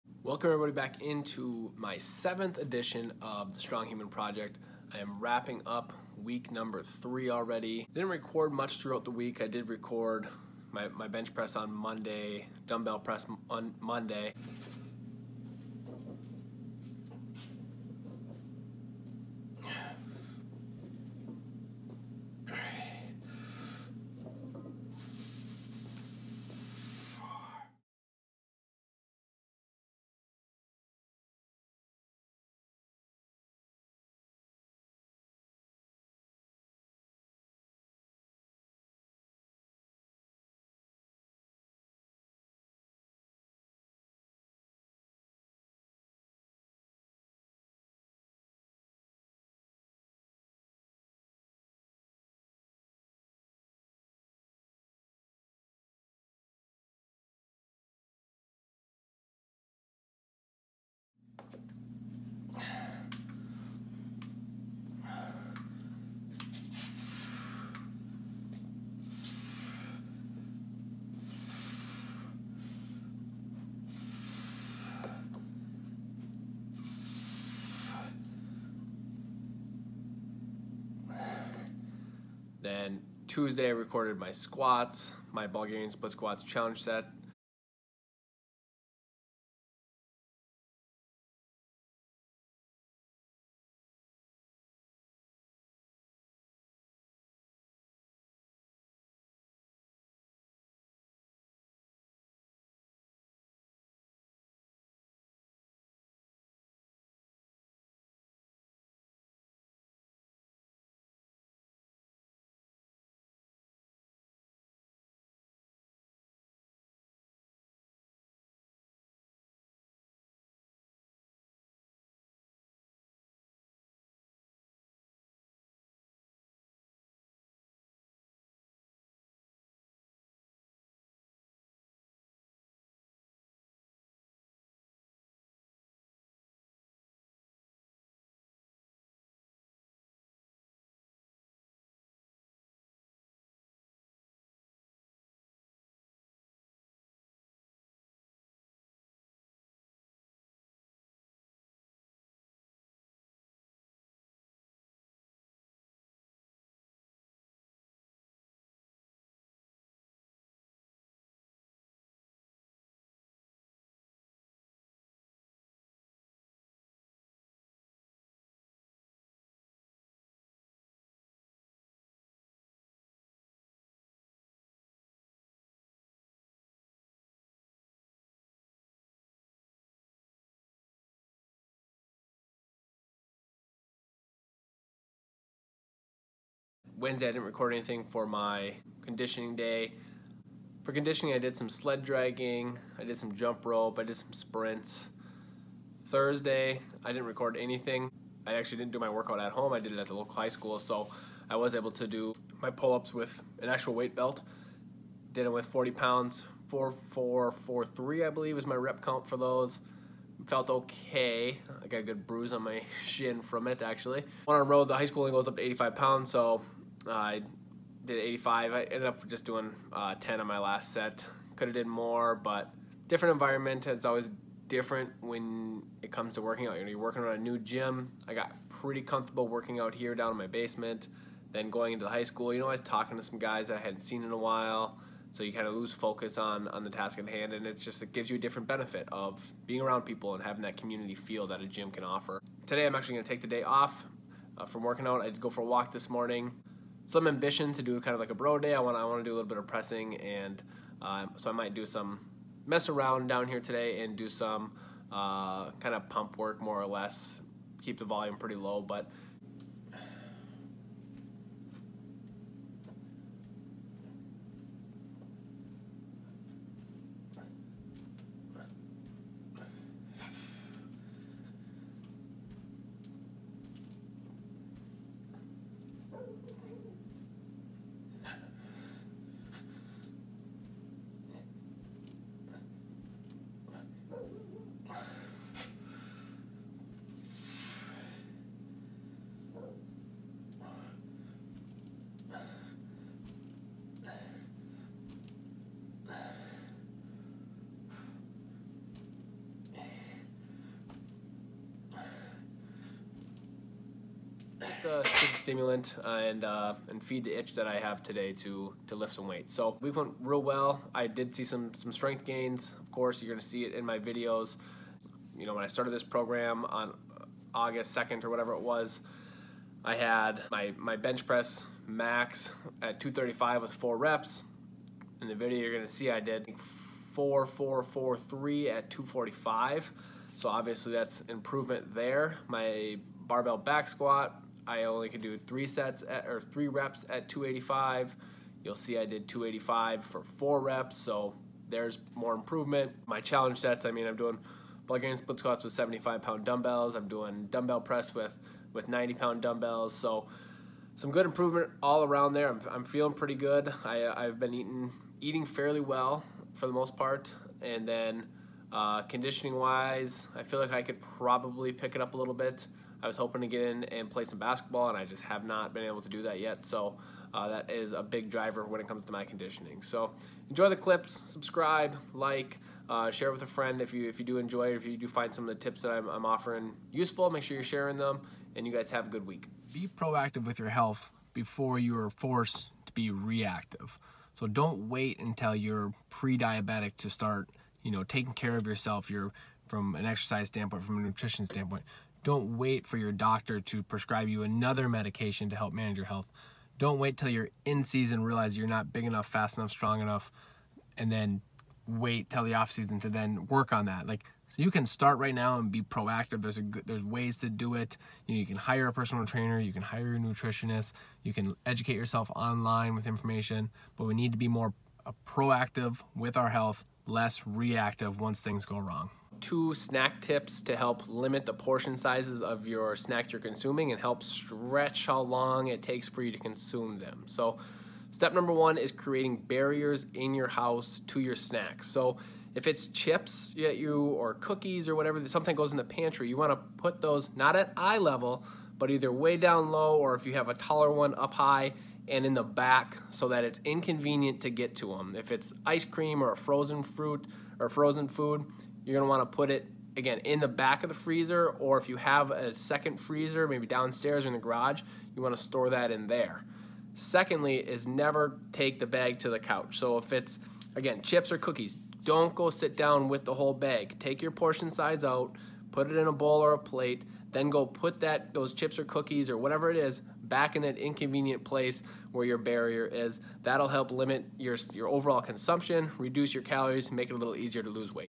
The Strong Human Project Episode #7: Week 3 Training Clips & Thoughts
Reflecting back on week 3. Strength is improving!